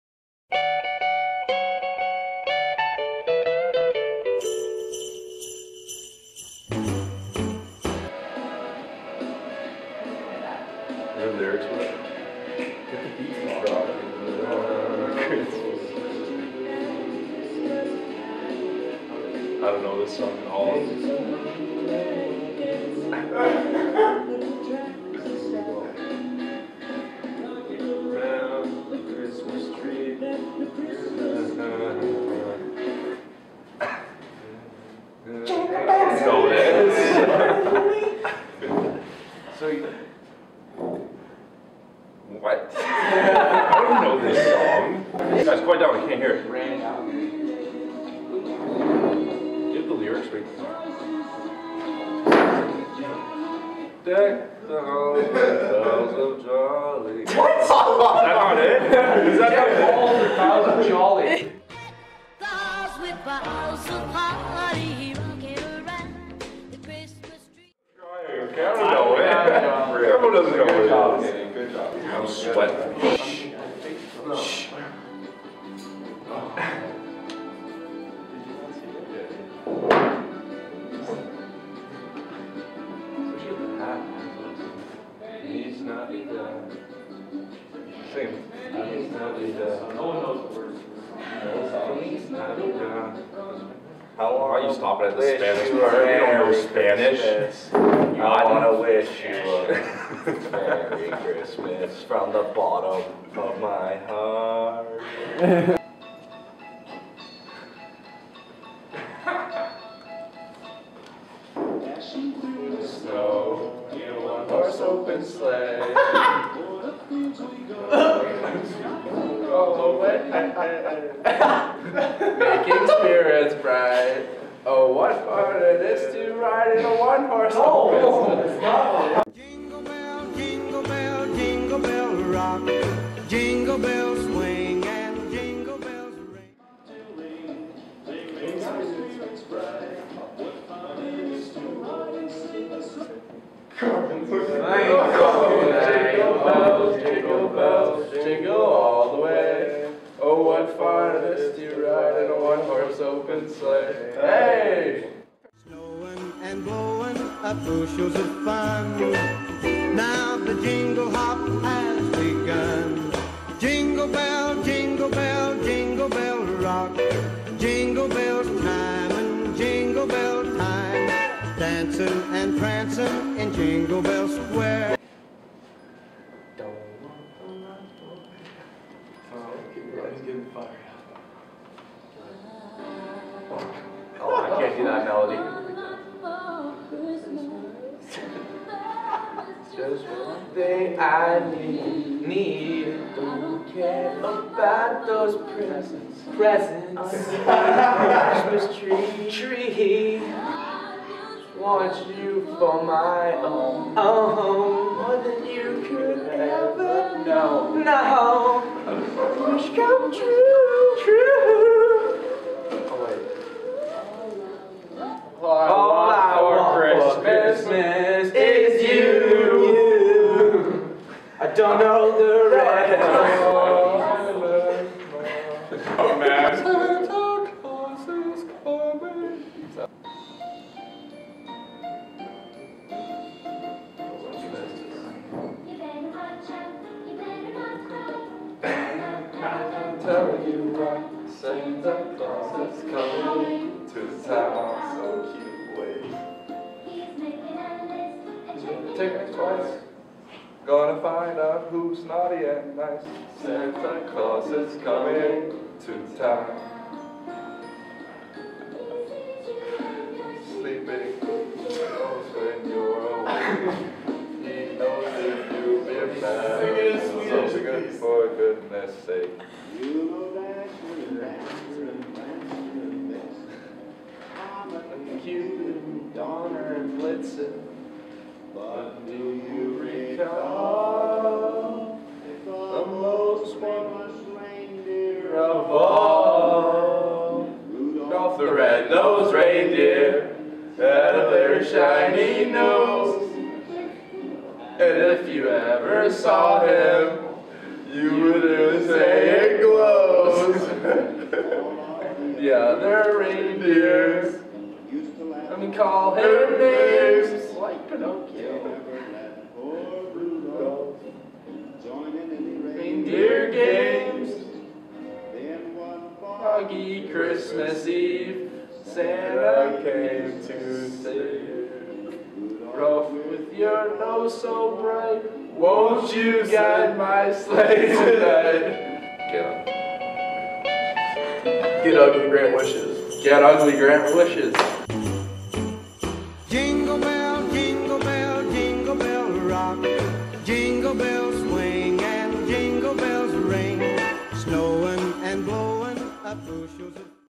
GetUglyGrantWishes
We asked our players to put on their ugly Christmas sweaters and sing some of the most well-known Christmas carols out there, all in support of Get Ugly Grant Wishes, in support of Children's Wish Foundation.

We invite you to wear your favourite ugly Christmas sweater to our game on Sunday, Nov. 7 when the Generals take on the Erie Otters at 6:05 PM.